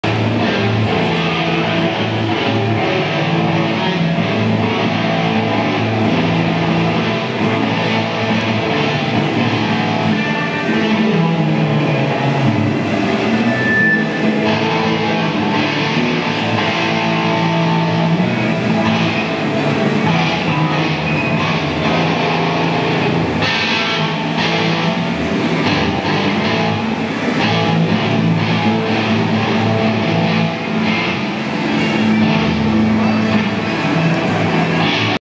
NY Amp Show - May 09 Sounds from the lobby
May not care for this -  its just to give you an idea of what it was like just standing in the hallway